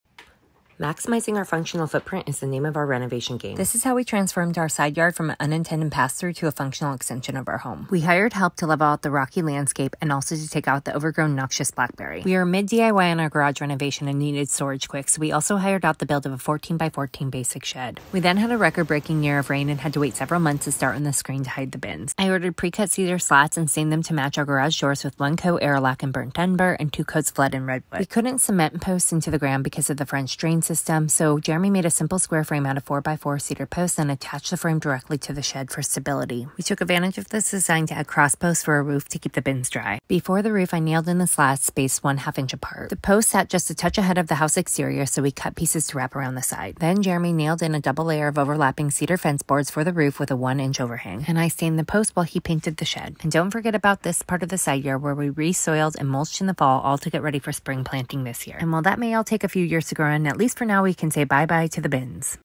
Transform unattended passthrough side yard into a functional and cohesive extension of your home
Maximizing our functional footprint is basically the name of our renovation game.  This is how we transformed our side yard from an unattended passthrough to a functional and cohesive extension of our home:

- Leveled out the side yard
- Added a 14x14 storage shed that fits power tools, garden tools, paint and home improvement supplies and a garden potting station
- Built a privacy screen to hide the bins, anchored to the shed structure with a roof to keep bins dry
- Stained privacy screen same color as garage + painted shed same color as house exterior
- Soiled, mulched and replanted the side yard

PRECUT CEDAR SLATS DELIVERED DOORSTEP